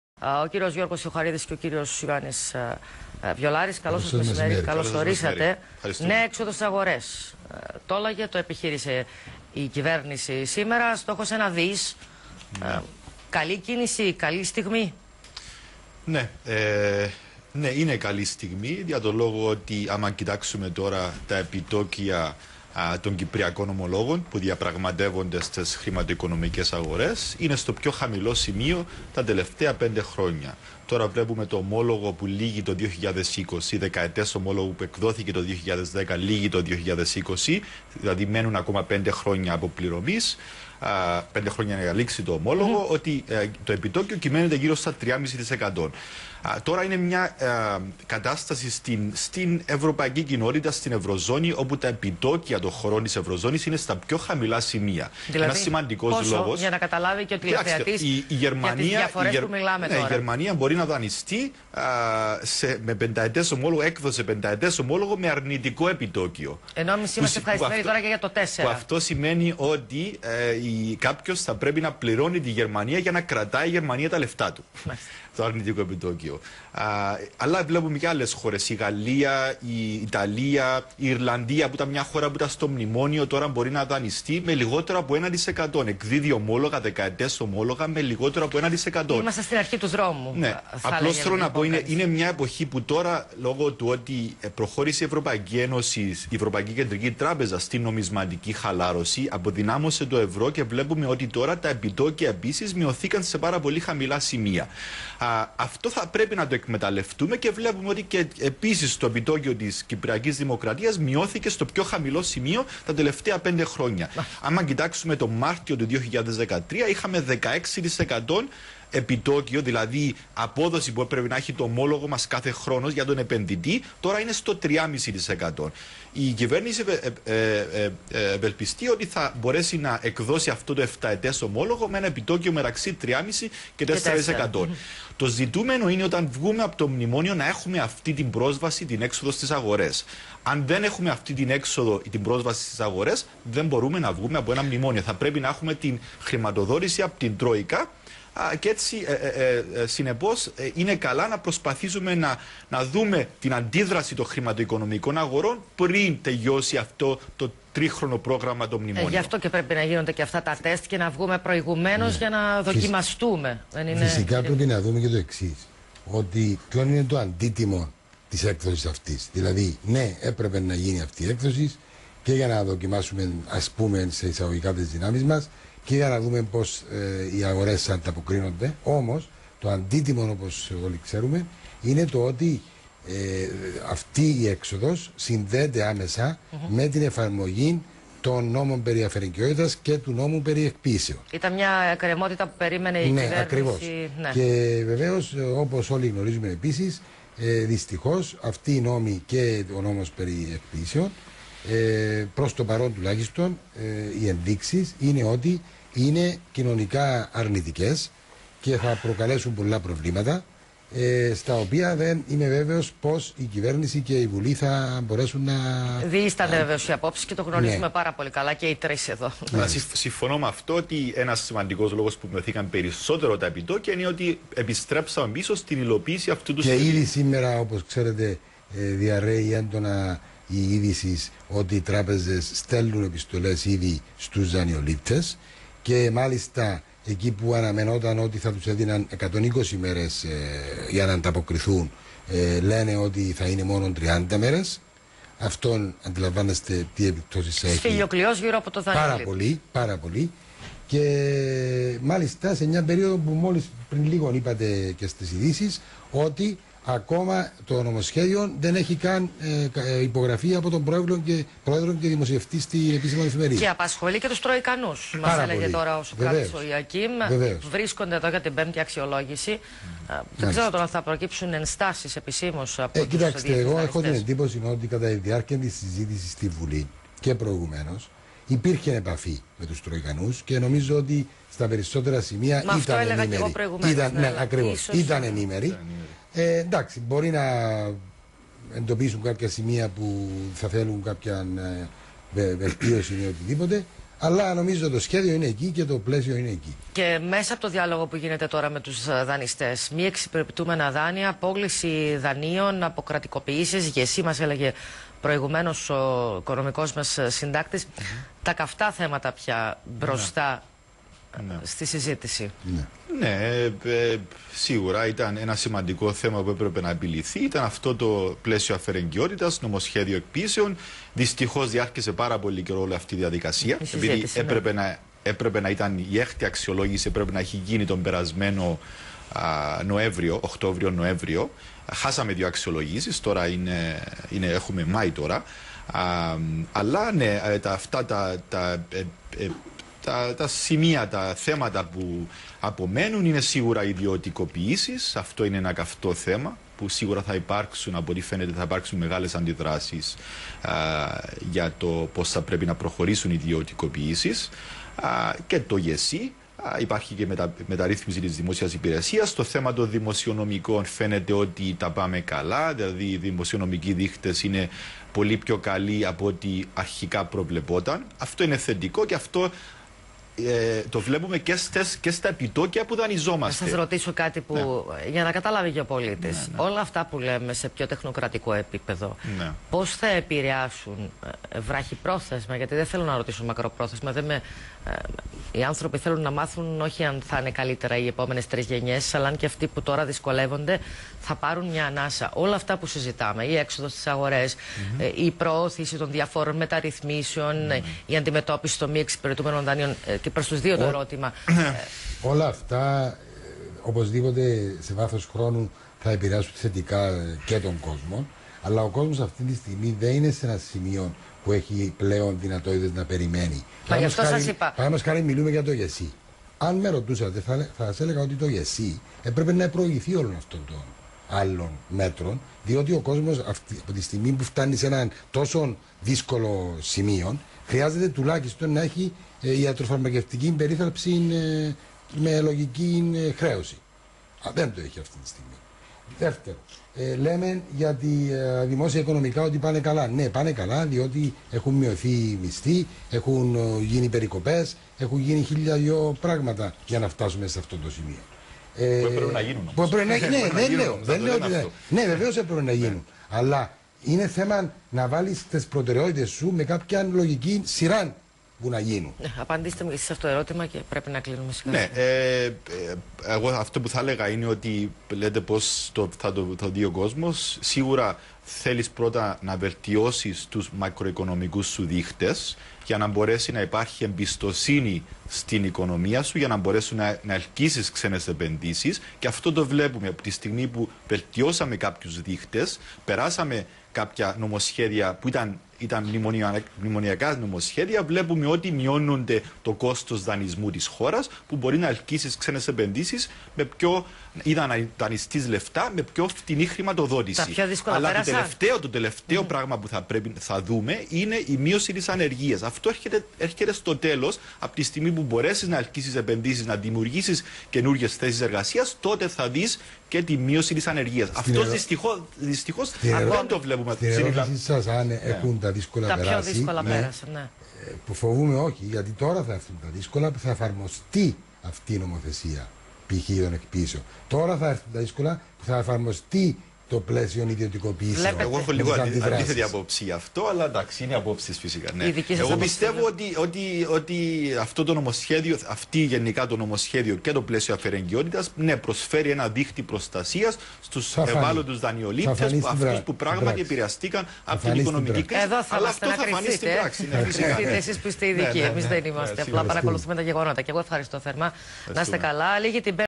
Ο Δρ. Γιώργος Θεοχαρίδης συζητά για την κάθοδο της Τρόικας στην Κύπρο
Ο Δρ. Γιώργος Θεοχαρίδης συζητά για την κάθοδο της Τρόικας στην Κύπρο και την τελευταία έξοδο της Κυπριακής κυβέρνησης στις χρηματοοικονομικές αγορές